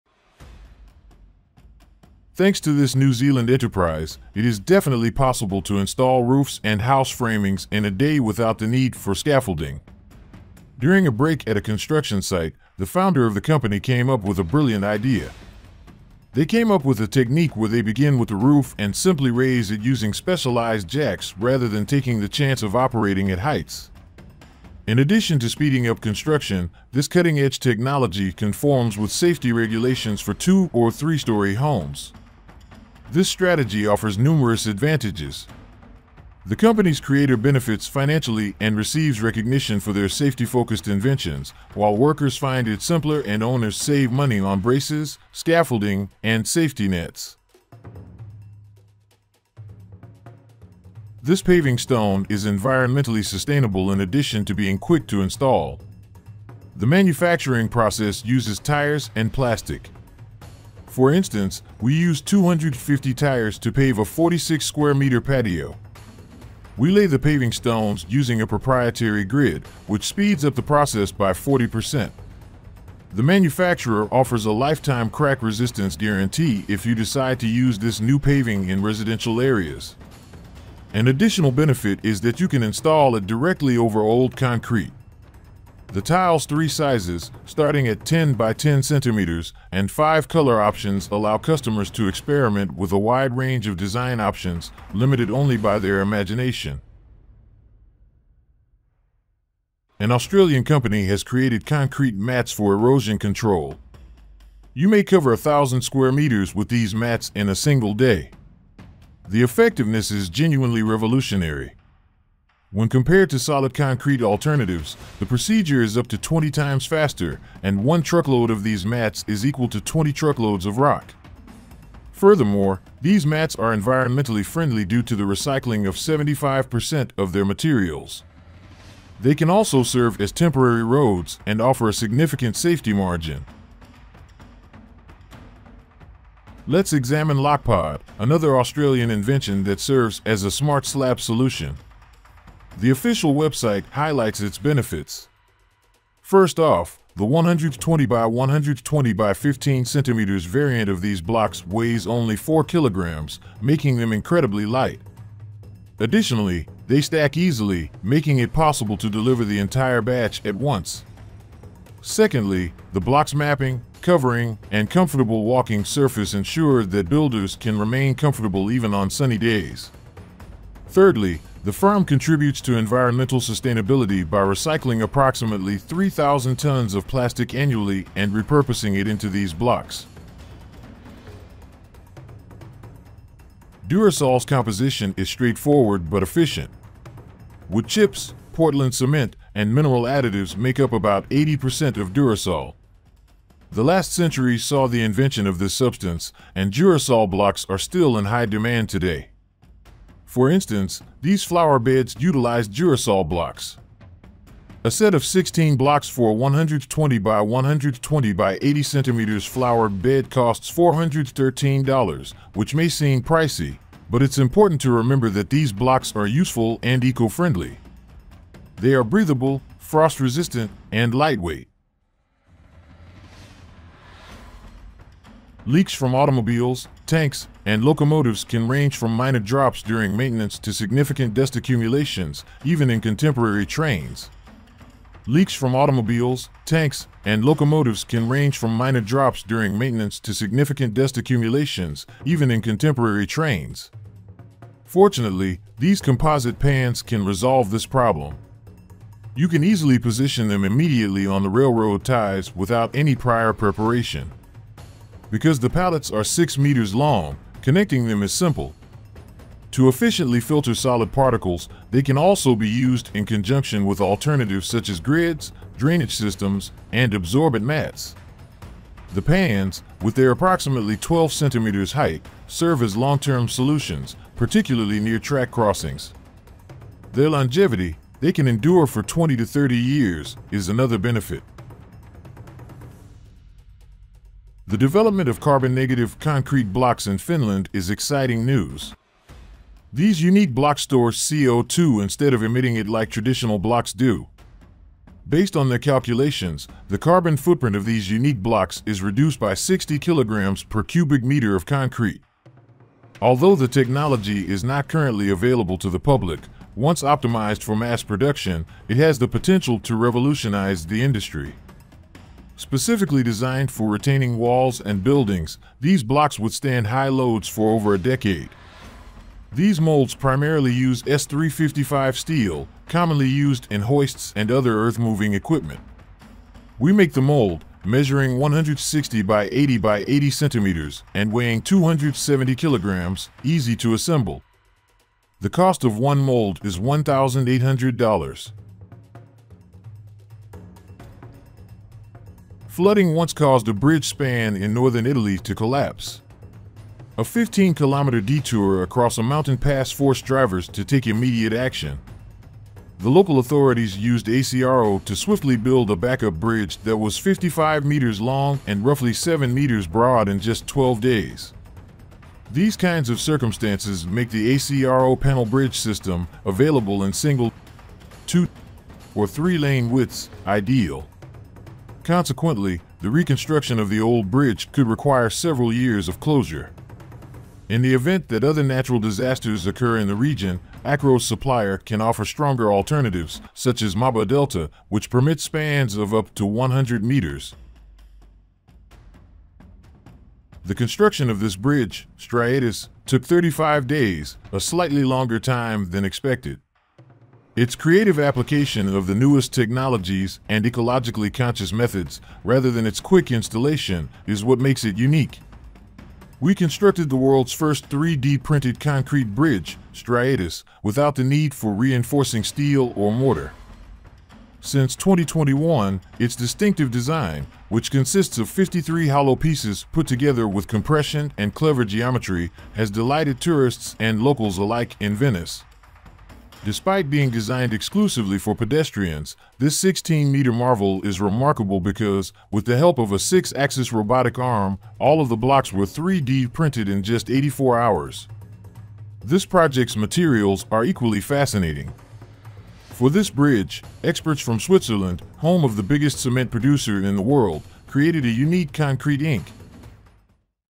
Mind Blowing Construction Technologies You Should See
BUILD AND LIFT.
Thanks to this New Zealand enterprise, it is definitely possible to install roofs and house framings in a day without the need for scaffolding. During a break at a construction site, the founder of the company came up with a brilliant idea.

AZEK PAVERS.
This paving stone is environmentally sustainable in addition to being quick to install. The manufacturing process uses tires and plastic. For instance, we used 250 tires to pave a 46-square-meter patio.

EARTHLOK.
An Australian company has created concrete mats for erosion control. You may cover a thousand square meters with these mats in a single day. The effectiveness is genuinely revolutionary.

LOKPOD.
Let's examine Lockpod, another Australian invention that serves as a smart slab solution. The official website highlights its benefits. First off, the 120 x 120 x 15 cm variant of these blocks weighs only 4 kg, making them incredibly light.

LEIER DURISOL.
Durisol's composition is straightforward but efficient. Wood chips, Portland cement, and mineral additives make up about 80% of Durisol. The last century saw the invention of this substance, and Jurisol blocks are still in high demand today.

RAILROAD SPILL CONTAINMENT.
Leaks from automobiles, tanks, and locomotives can range from minor drops during maintenance to significant dust accumulations, even in contemporary trains. Leaks from automobiles, tanks, and locomotives can range from minor drops during maintenance to significant dust accumulations, even in contemporary trains.

BETONBLOCK.
The development of carbon-negative concrete blocks in Finland is exciting news. These unique blocks store CO2 instead of emitting it like traditional blocks do. Based on their calculations, the carbon footprint of these unique blocks is reduced by 60 kg per cubic meter of concrete.

ACROW 700XS.
Flooding once caused a bridge span in northern Italy to collapse. A 15-kilometer detour across a mountain pass forced drivers to take immediate action. The local authorities used ACRO to swiftly build a backup bridge that was 55 meters long and roughly 7 meters broad in just 12 days.

STRIATUS.
The construction of this bridge, Striatus, Stray Artist, took 35 days, a slightly longer time than expected. Its creative application of the newest technologies and ecologically conscious methods—rather than its quick installation—is what makes it unique.